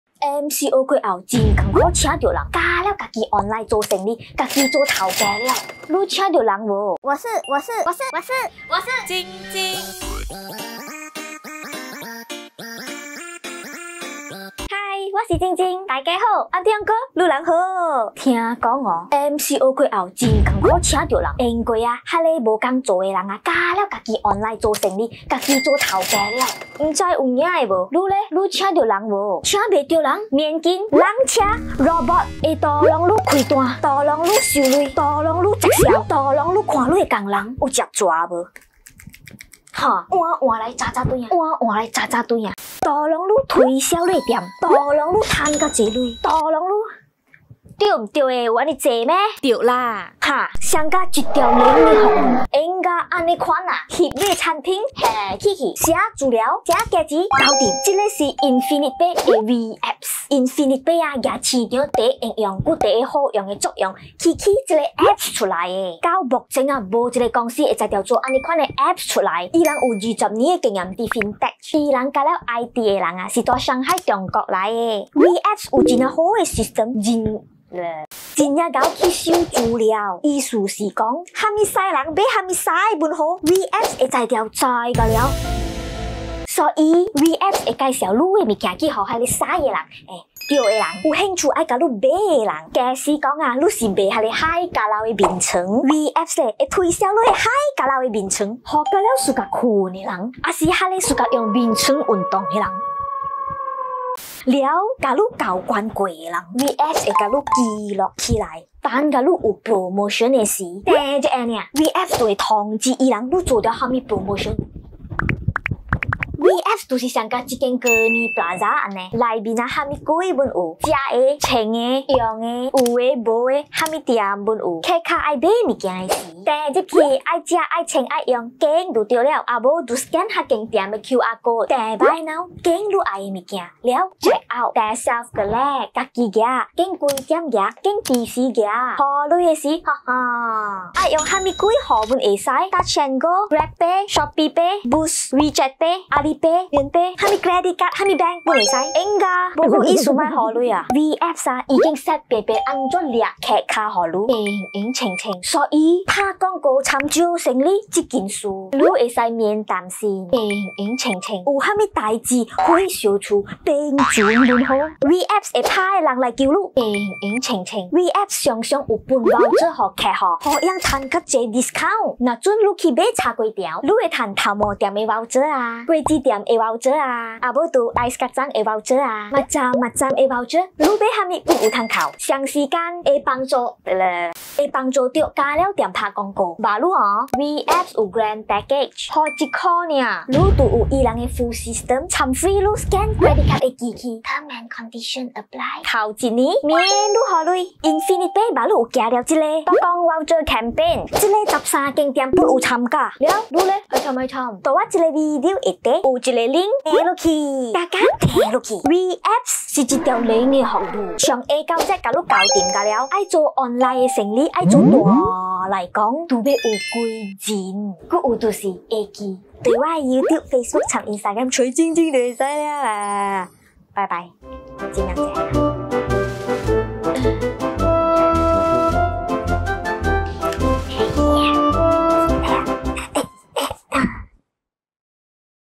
Handle Business in easy way - Infinitepay 简单处理大生意
Merchant can manage their online store just with handphone.
Can receive payment with any E-Wallet
-TouchNGo , GrabPay, Boost, WechatPay or Maybank
- Most Bank and Credit Card are acceptable

Merchant will have all the features
- Self Upload product and info instantly
- Check Order
- Check Payment
- Check Transaction
- Staff Clock in & out System

WIIW APPS have Grant Package
Pay RM1 to get a package with full system + free scan credit card eh payment machine.
*tnc applied

Powered by ONSMO LIGHTING
RGB LIGHT ⬇️

Talent 演员：
Jing Jing 妗妗